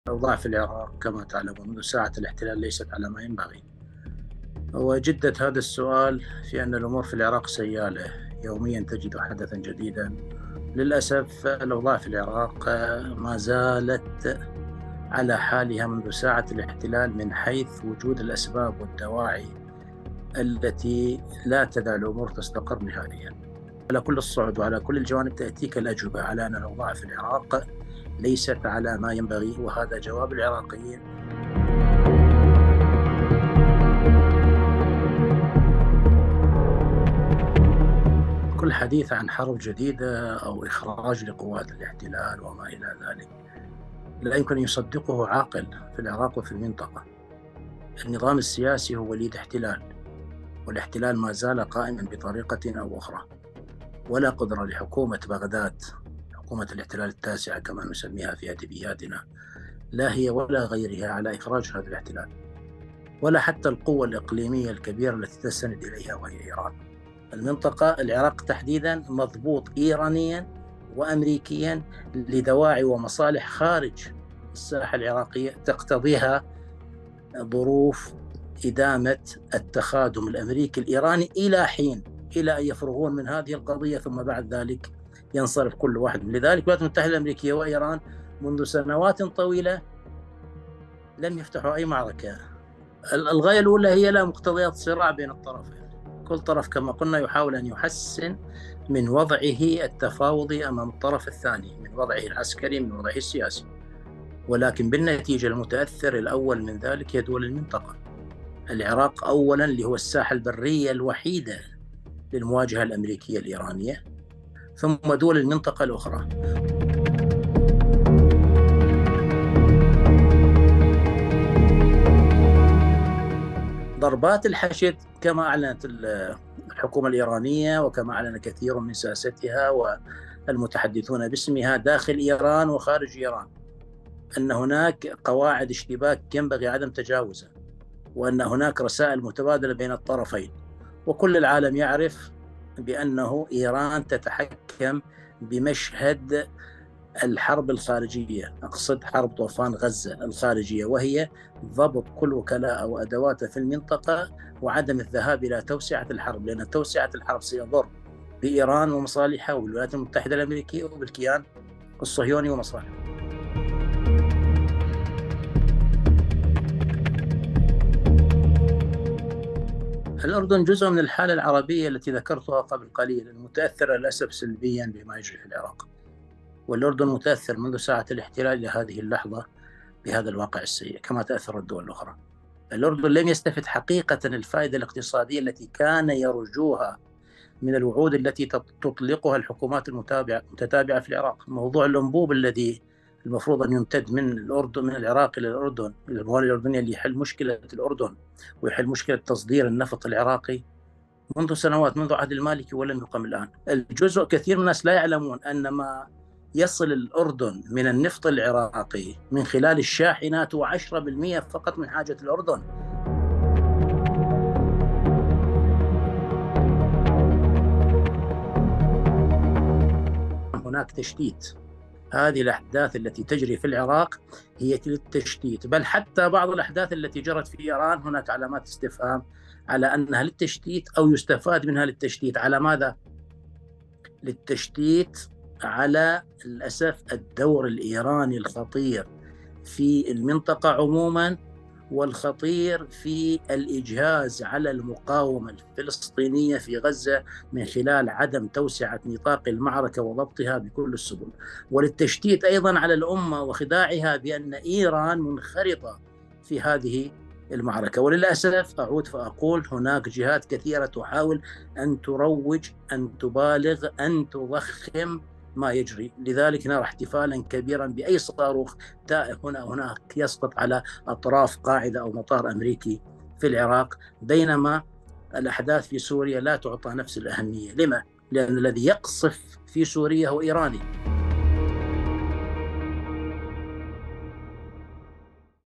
تطورات الأحداث في العراق.. ما هي تبعاتها على الأردن؟
مع تصاعد التوتر في العراق بين فصائل مسلحة تعتبرها سلطات بغداد قوى أمن رسمية، والقوات الأمريكية، على خلفية الحرب الإسرائيلية على غزة، ترتفع حدة مخاوف دول المنطقة من تداعيات تلك المواجهات، ونشوب أزمة جديدة تفاقم حجم الأعباء عليها.

التطورات اللافتة في العراق، تفرض تحديا جديدا على دول الجوار، خاصة الأردن، لا سيما أن المملكة تعقد آمالا على جارها الشرقي، وتعتمد في سد جزء من احتياجاتها النفطية على ما يأتيه منه، فضلاً عن تحالف مفترض يجمعهما مع مصر، بات يعرف بـ"تحالف الشام".

الأناضول أجرت مقابلة عبر تطبيق "زوم" مع مسؤول المكتب السياسي في هيئة علماء المسلمين بالعراق مثنى حارث الضاري، تحدث خلالها عن واقع بلاده، والآثار المتوقعة على دول المنطقة.